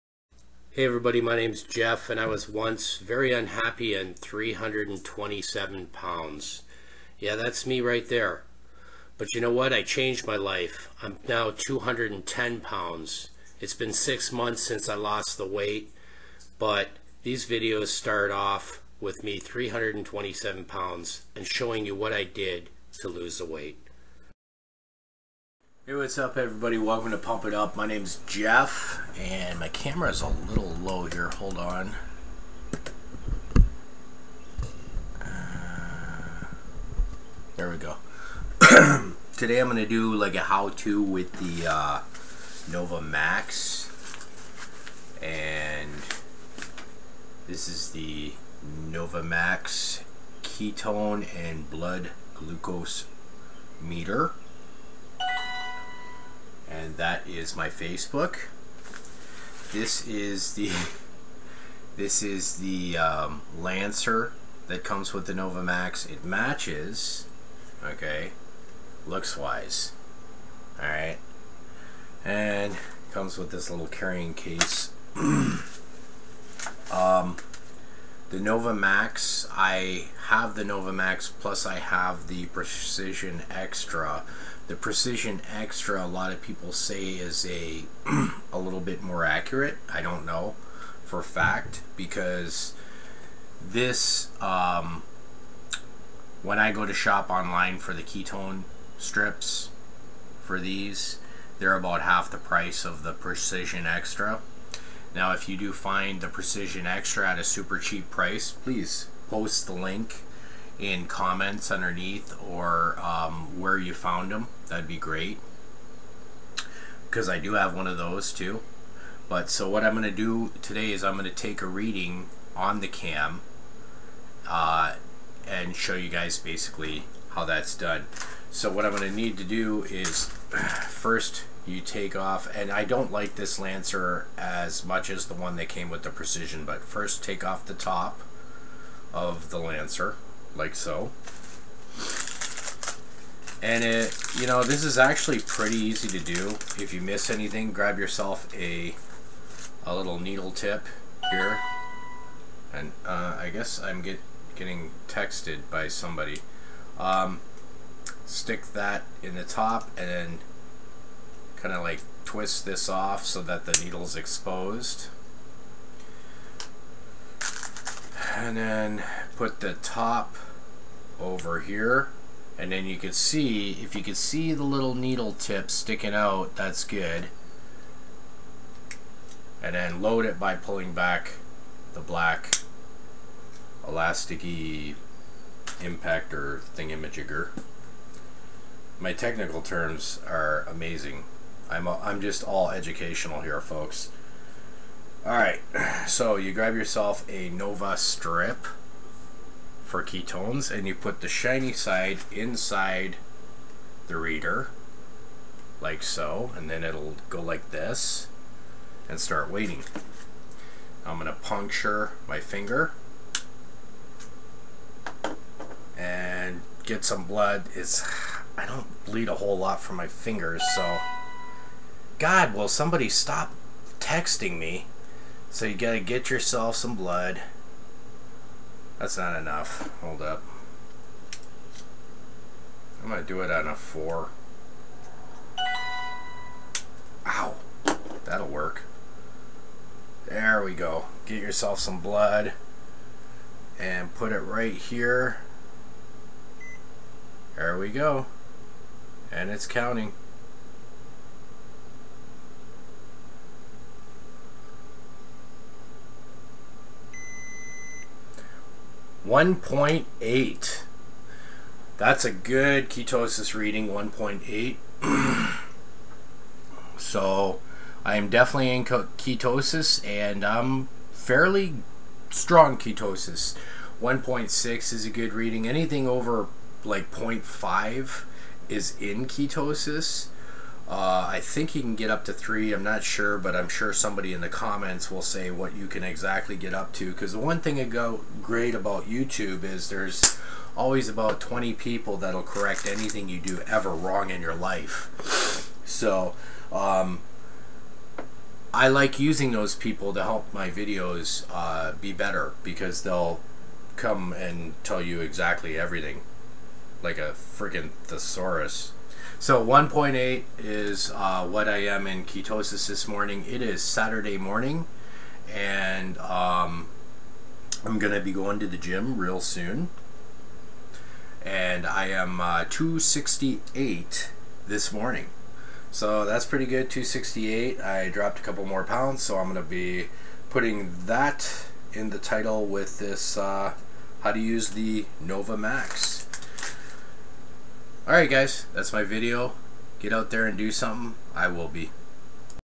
Nova Max -How to-
This is a little how to to check your ketone readings in your blood also I'm 162 pounds